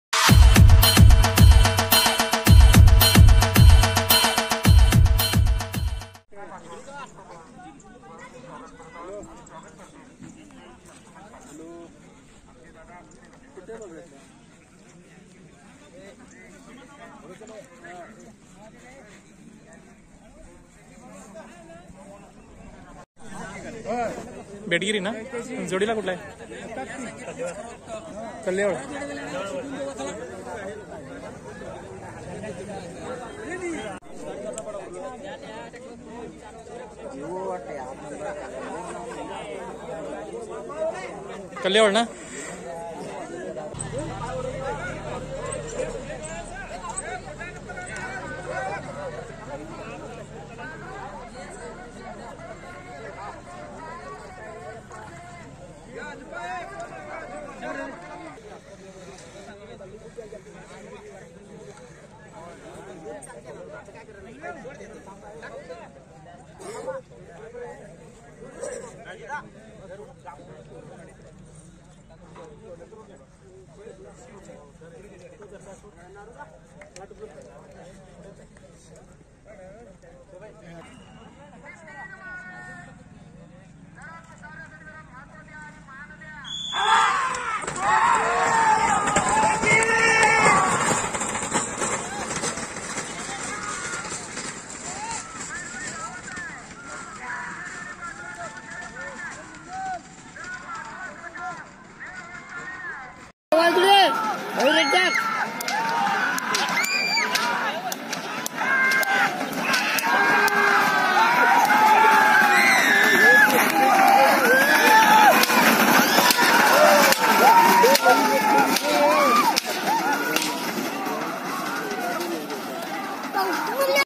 || Racing Bulls of Betgiri and Kallehol Running in Bailgada Sharyat ||
In this video you can watch racing bulls of Kallehol and Betgiri running in billock cart race at Kuppatgiri Khanapur.